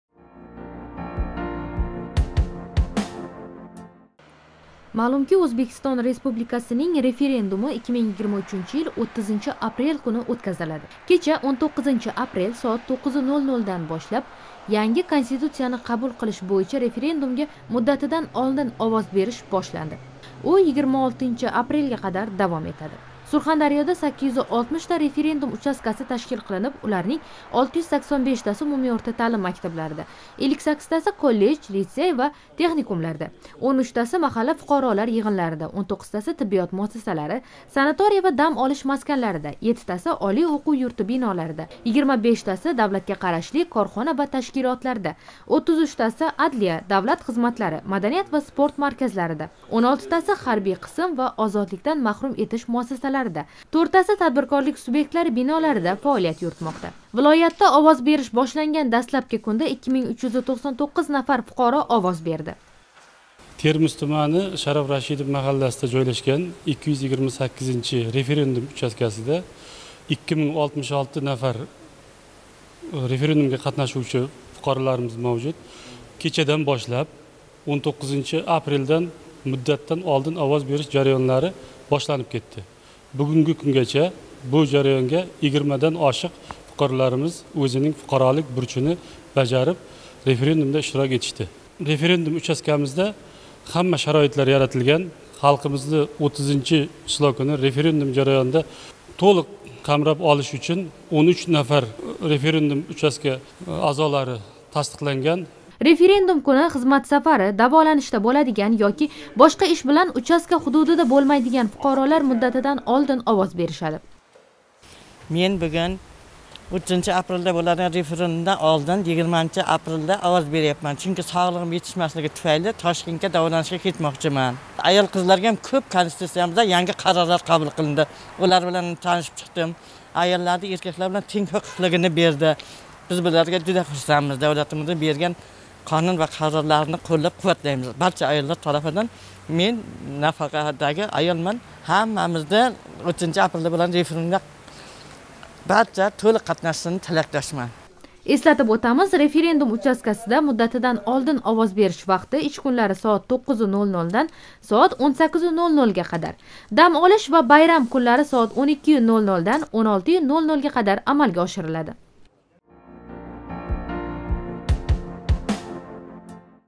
Фуқаролар референдумда муддатидан олдин овоз беришмоқда
Маълумки, Ўзбекистон Республикасининг референдуми 2023 йил 30 апрель куни ўтказилади.

Кеча, 19 апрель соат 9:00дан янги Конституцияни қабул қилиш бўйича референдумга муддатидан олдин овоз бериш бошланди. У 26 апрелга қадар давом этади.

Сурхондарёда овоз бериш бошланган дастлабки кунда 2399 нафар фуқаро овоз берди.